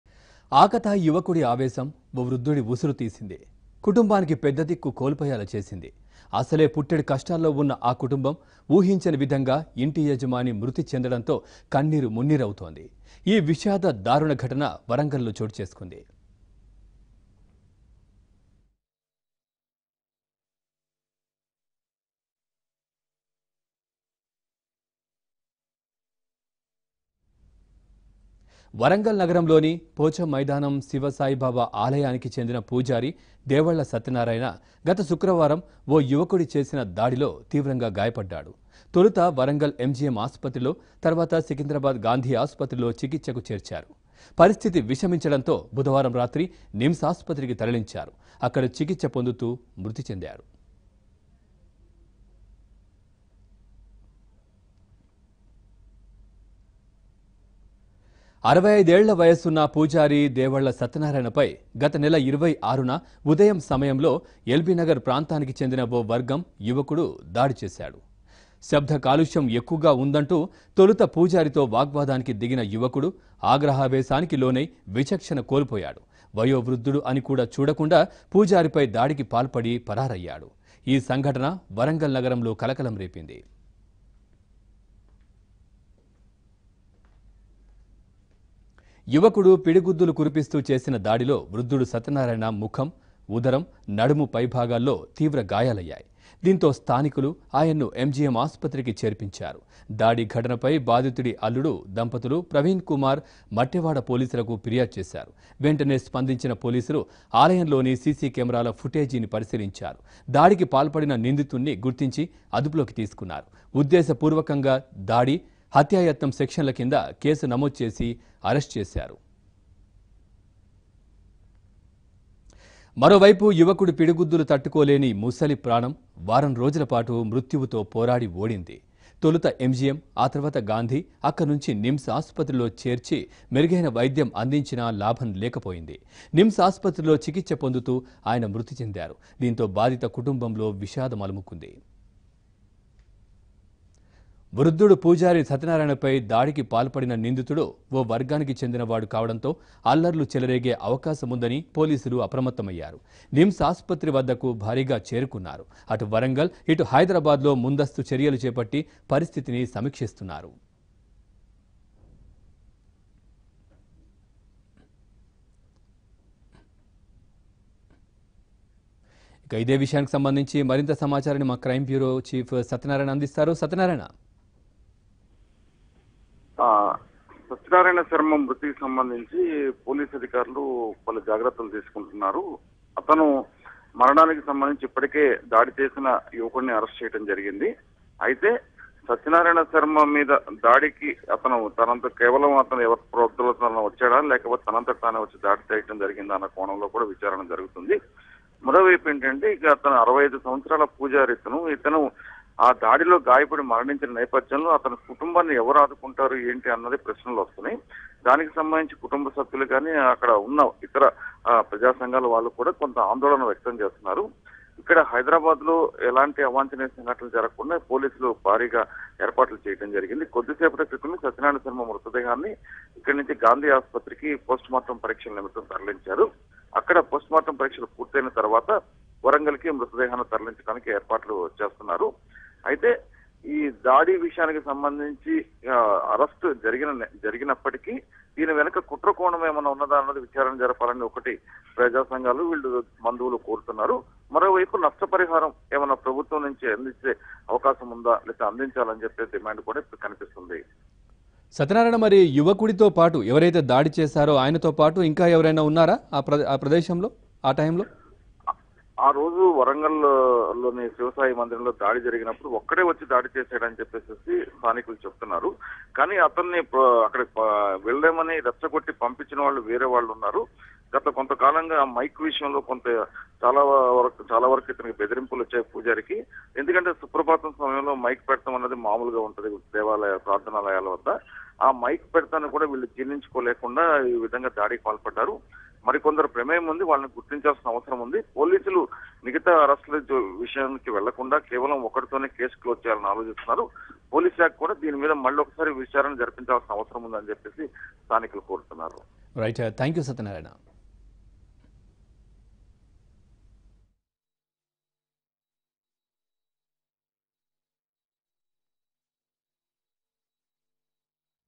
Sai Baba Temple Priest lost life after attack in Warangal | ABN Telugu
Sai Baba Temple priest lost life after attack in Warangal. The priest Sathyanarayan got assaulted by a youth when they crossed the words for the sound pollution from the temple.